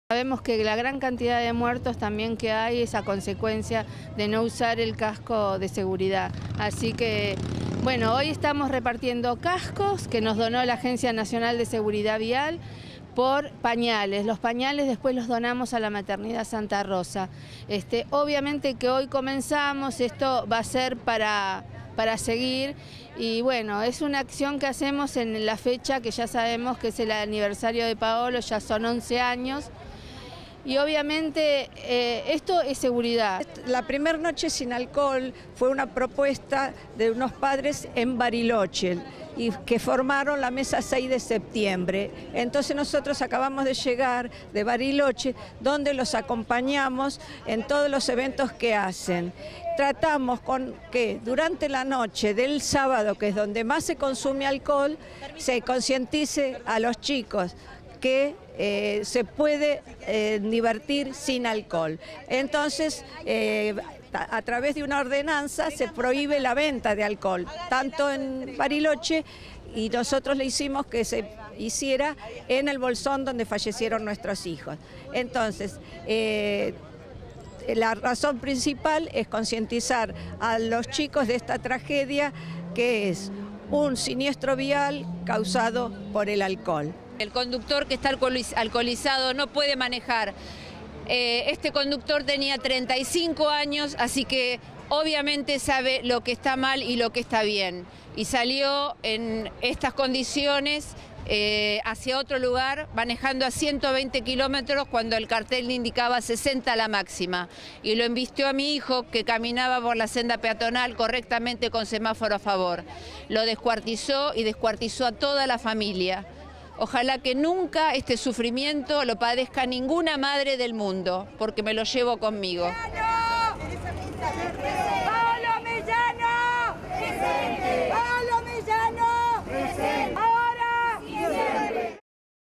HOMENAJE PARA CONCIENTIZAR
Familiares de víctimas de tránsito entregaron
cascos en Avenida del Libertador y Corrientes
en Vicente López, para concientizar a la población
sobre la seguridad vial. La iniciativa fue llevada
adelante por la mamá de Paolo Mellano, atropellado
en el 2003. Repintaron la estrella amarilla que
lo recuerda.